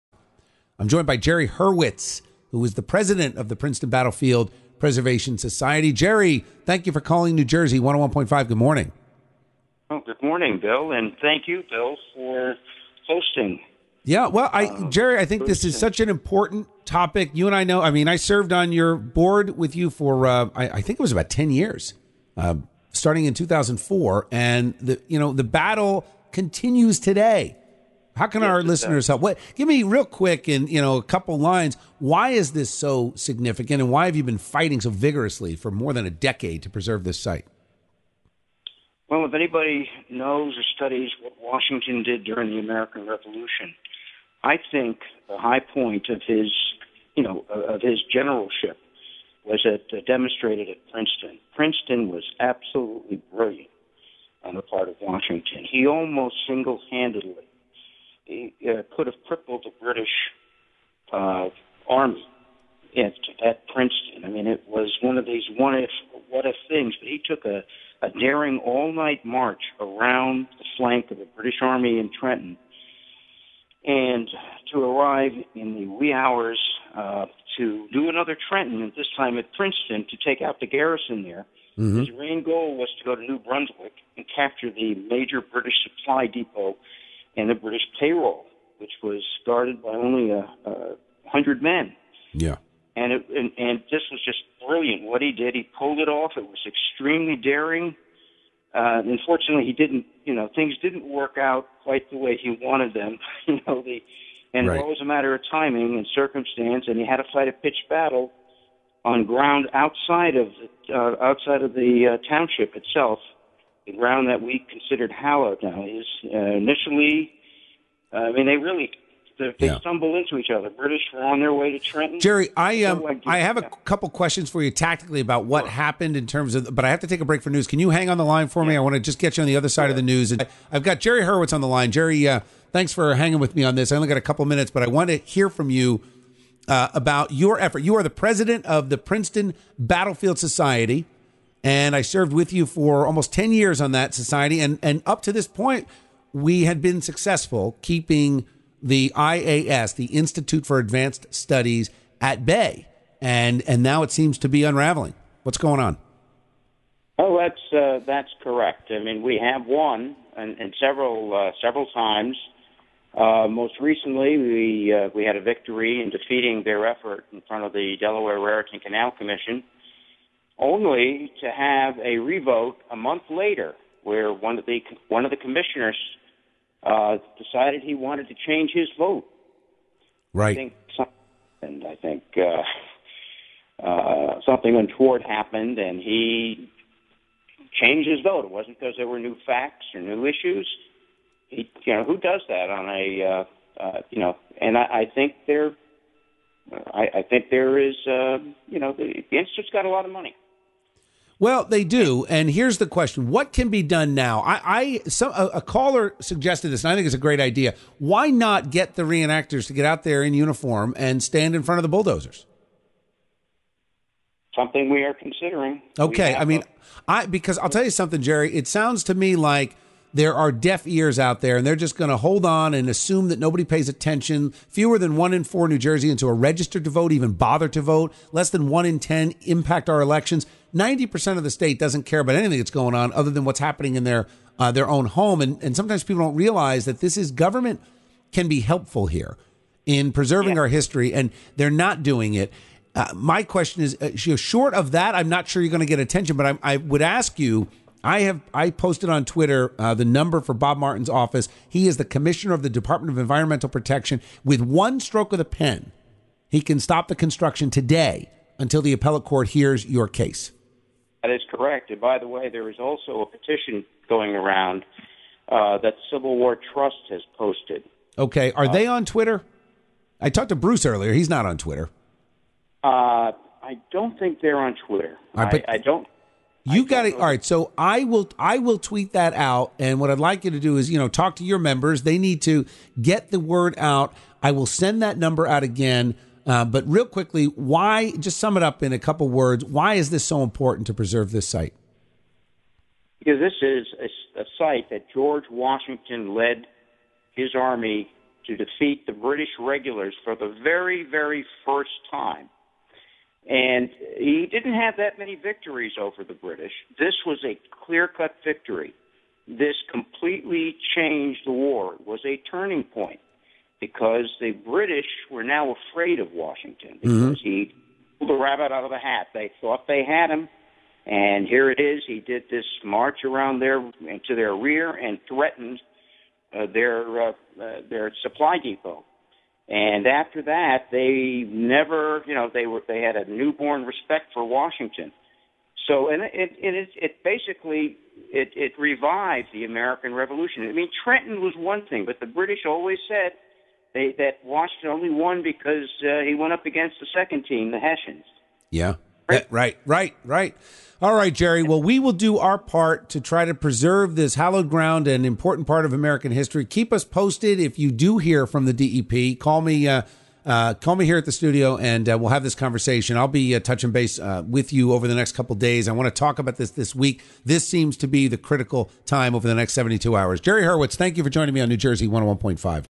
Defending historic Princeton Battlefield
Princeton Battlefield Society President Jerry Hurwitz promotes preservation of the historic landmark.

(Photo by: @S_K_Wilson)